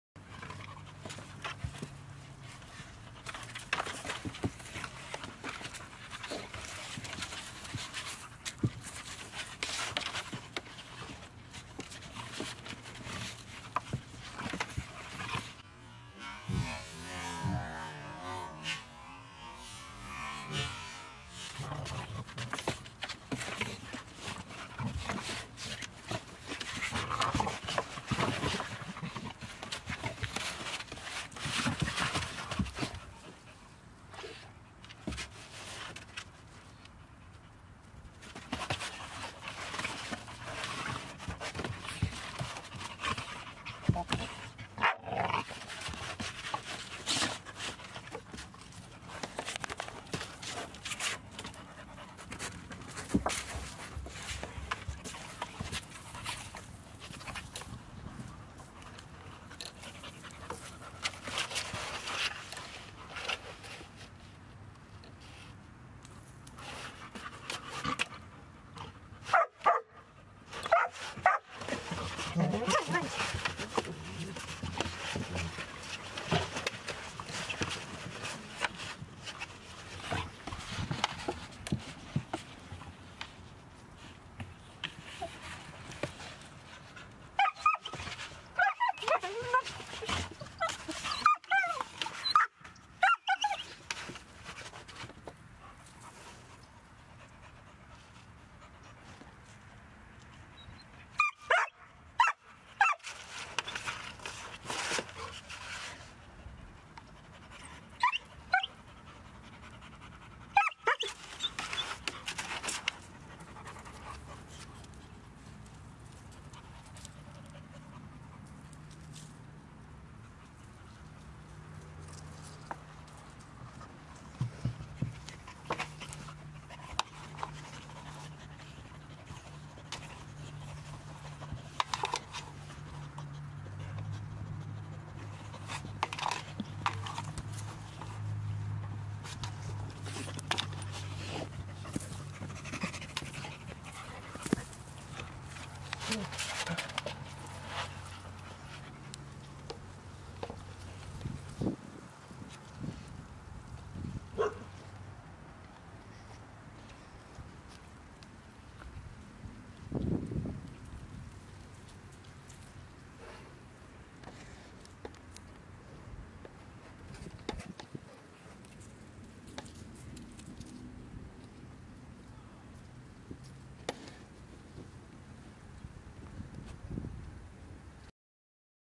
Weimaraner and Pit Bull Play in Tunnels
Talli and Pearl duke it out in the tunnel obstacles.